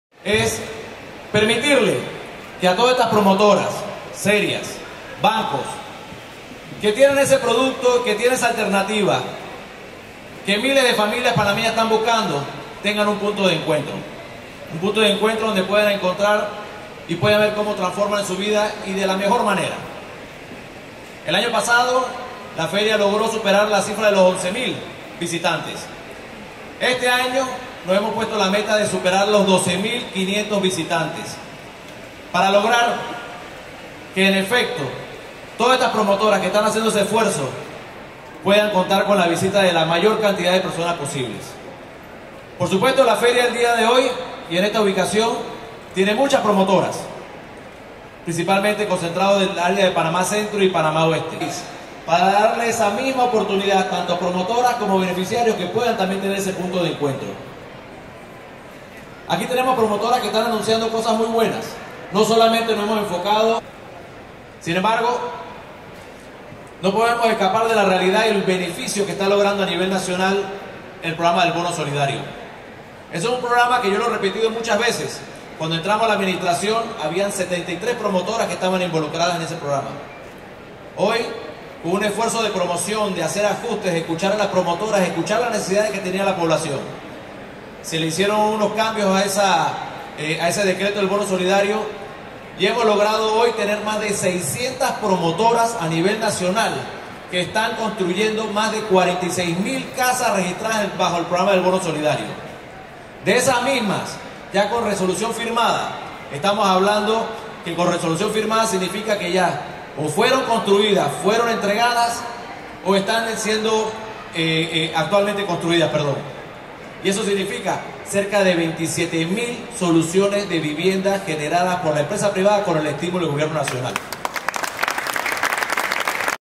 Arrancó Expo- Mivi 2017
Se esperan que más de 12 mil personas visiten la feria para conocer de las facilidades para obtener su vivienda, dijo en el acto de inauguración el titular de la cartera de Vivienda Mario Etchelecu